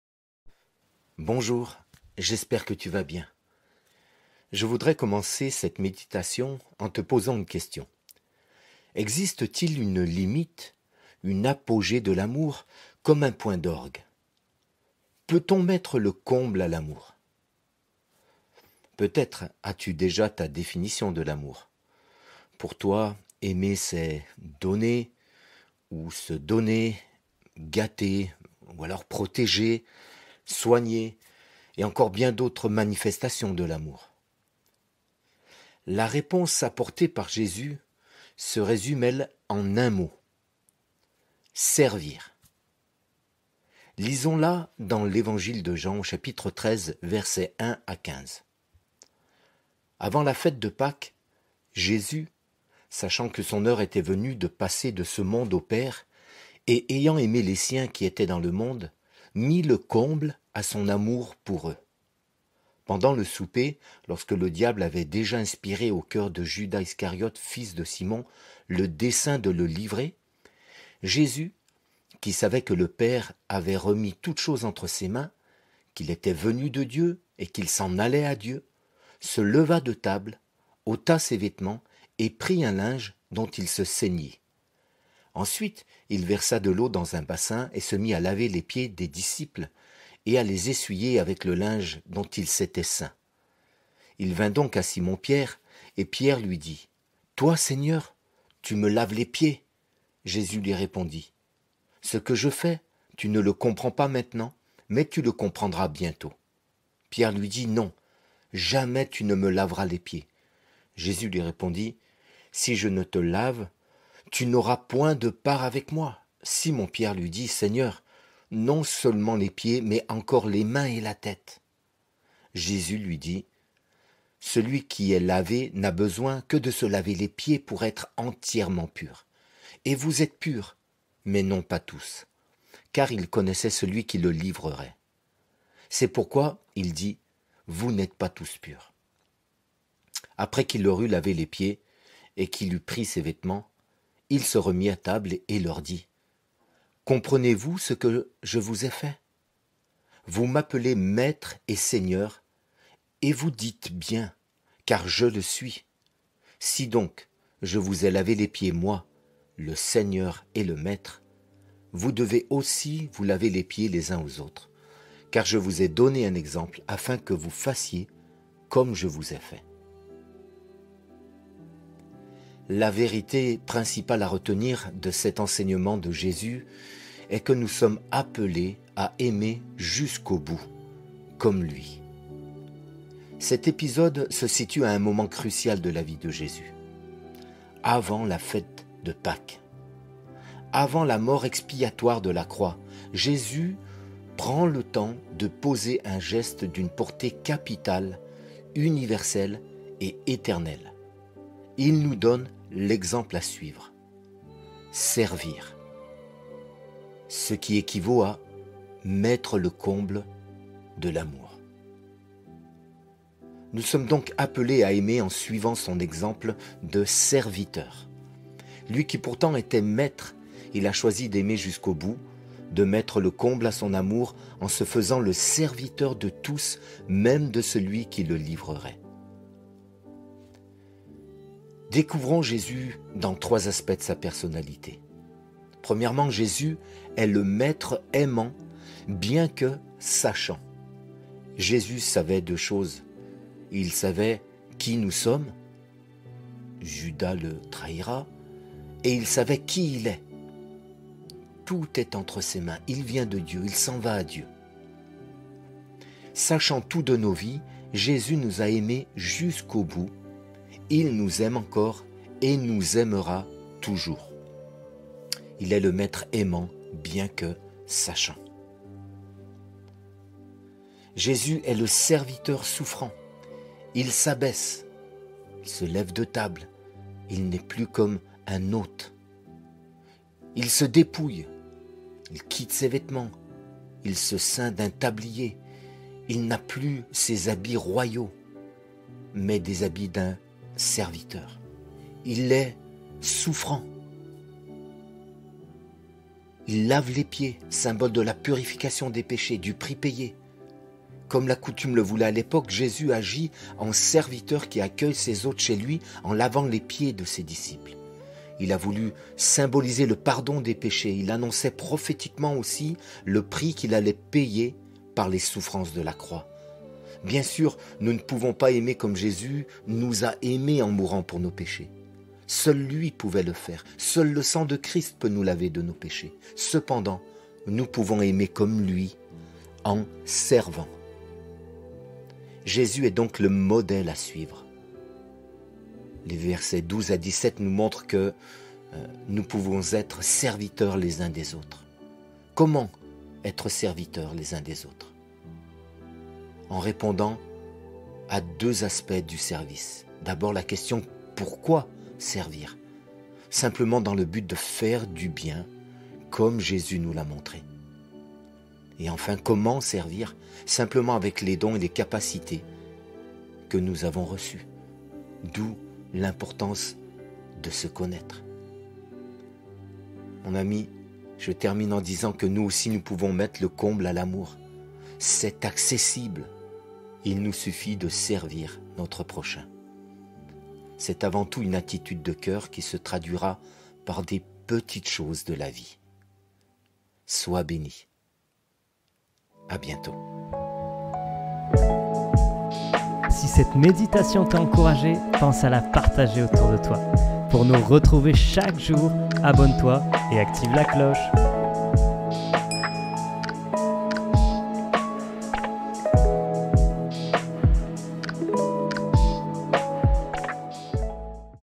SERVIR, LE COMBLE DE L’AMOUR ! / L'Évangile du Jour avec Tony Tornatore
Aujourd'hui nous méditons sur ces paroles du Christ :

"Avant la fête de Pâque, Jésus, sachant que son heure était venue de passer de ce monde au Père, et ayant aimé les siens qui étaient dans le monde, mit le comble à son amour pour eux.
Pendant le souper, lorsque le diable avait déjà inspiré au coeur de Judas Iscariot, fils de Simon, le dessein de le livrer,
Jésus, qui savait que le Père avait remis toutes choses entre ses mains, qu'il était venu de Dieu, et qu'il s'en allait à Dieu, se leva de table, ôta ses vêtements, et prit un linge, dont il se ceignit.
Ensuite il versa de l'eau dans un bassin, et il se mit à laver les pieds des disciples, et à les essuyer avec le linge dont il était ceint.
Il vint donc à Simon Pierre ; et Pierre lui dit : Toi, Seigneur, tu me laves les pieds !
Jésus lui répondit : Ce que je fais, tu ne le comprends pas maintenant, mais tu le comprendras bientôt. Pierre lui dit : Non, jamais tu ne me laveras les pieds. Jésus lui répondit : Si je ne te lave, tu n'auras point de part avec moi. Simon Pierre lui dit : Seigneur, non seulement les pieds, mais encore les mains et la tête.
Jésus lui dit : Celui qui est lavé n'a besoin que de se laver les pieds pour être entièrement pur ; et vous êtes purs, mais non pas tous. Car il connaissait celui qui le livrait ; c'est pourquoi il dit : Vous n'êtes pas tous purs.
Après qu'il leur eut lavé les pieds, et qu'il eut pris ses vêtements, il se remit à table, et leur dit : Comprenez-vous ce que je vous ai fait ? Vous m'appelez Maître et Seigneur ; et vous dites bien, car je le suis. Si donc je vous ai lavé les pieds, moi, le Seigneur et le Maître, vous devez aussi vous laver les pieds les uns aux autres ; car je vous ai donné un exemple, afin que vous fassiez comme je vous ai fait.” (Jean 13 : 1-15)

Notre titre du jour est : SERVIR, LE COMBLE DE L’AMOUR !

00:00 Introduction

00:48 Verset de l'Évangile du jour

02:50 Méditation par Tony Tornatore